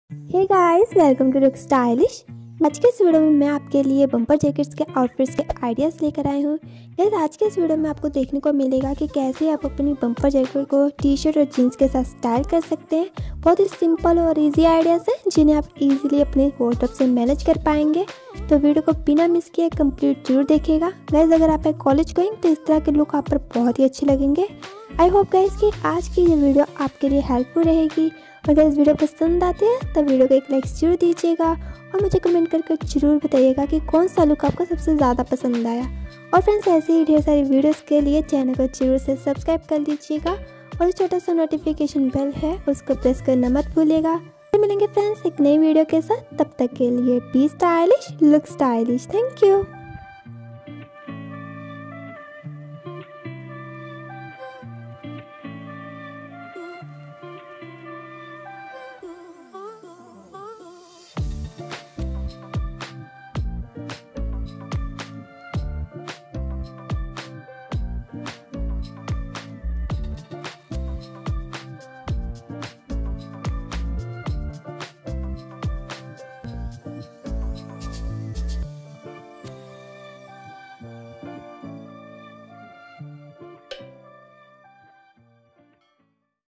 15 Bomber Jackets Outfit Ideas For Women || How To Style A Bomber Jackets || by Look Stylish
Hello friends 😊

15 Bomber Jackets Outfit Ideas For Women || How To Style A Bomber Jackets || by Look Stylish

About video :-
In today's video I gonna share bomber jackets outfit ideas for women, here you'll see different ways to style a ladies bomber jackets, I hope friends you like this video.